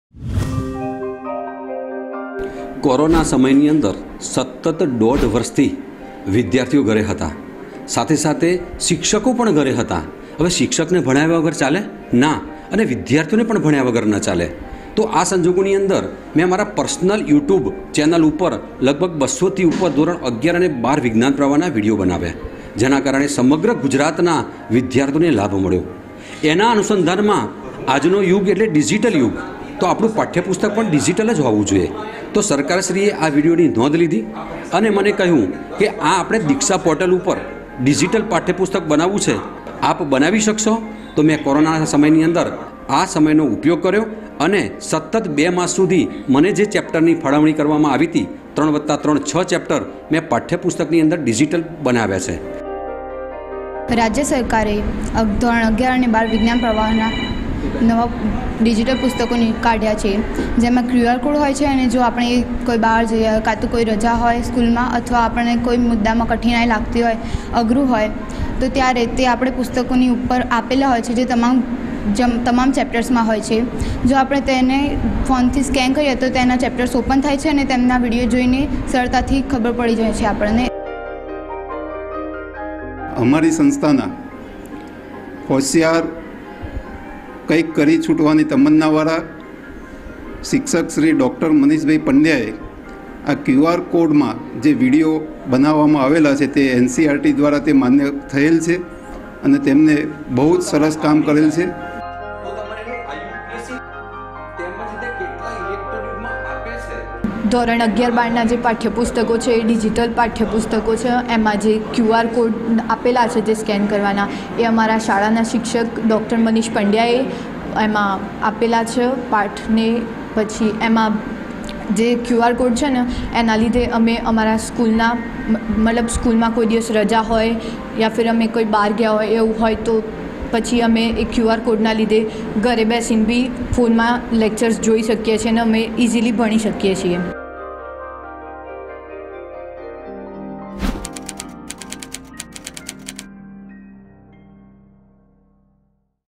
Online Teaching - ડો મનીશ પંડ્યાએ કમાલ કરી રસાયણશાસ્ત્રમાં તેમના યોગદાની સેન્ટર લેવલે લેવાઈ નોંધ
ડો મનીશ પંડ્યાએ કમાલ કરી

ગુજરાતના આ ડોક્ટરને સલામ
લોકડાઉનમાં બનાવ્યા વીડિયો
અઘરા શબ્દો પણ સાવ સરળ રીતે સમજાવ્યા
રસાયણશાસ્ત્રમાં તેમના યોગદાની સેન્ટર લેવલે લેવાઈ નોંધ